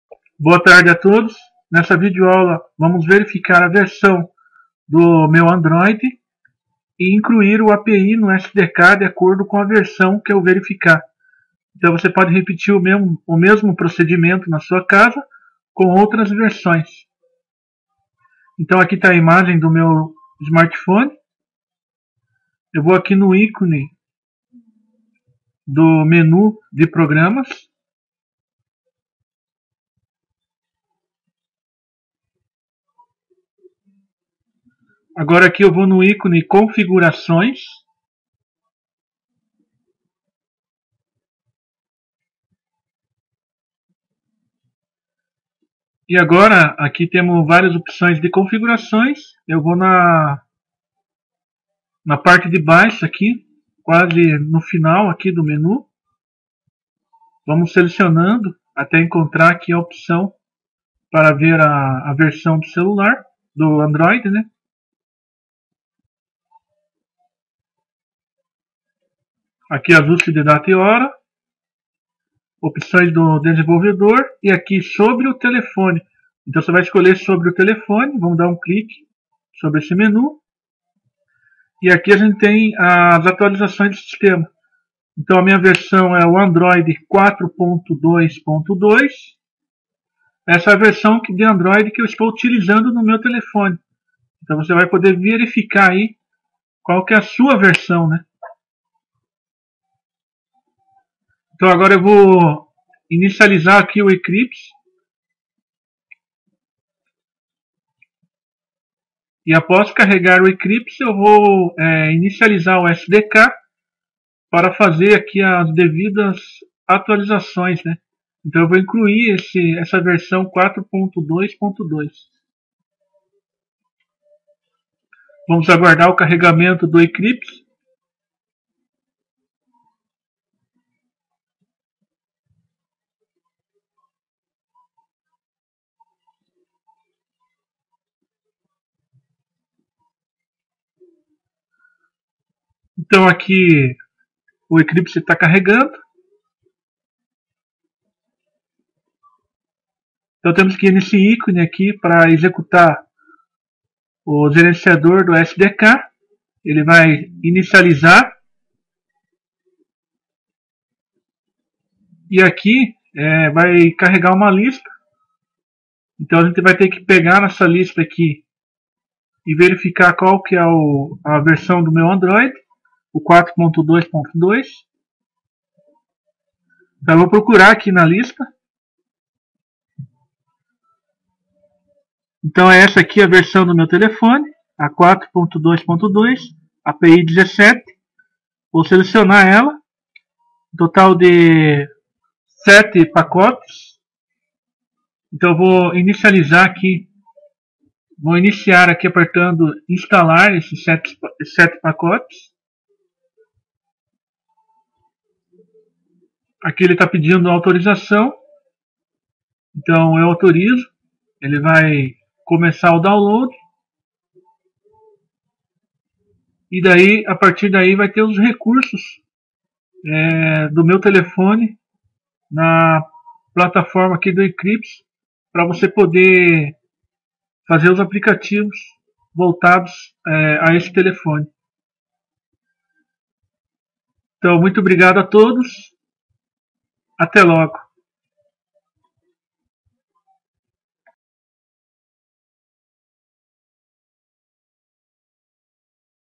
Verificando a versão Instalada do Android e incluindo API no SDK - Android - 150
Vídeo demonstrando como ver a versão do Android instalado em seu smartphone. E também como acrescentar API da versão no SDK Manager no Eclipse Luna 4.4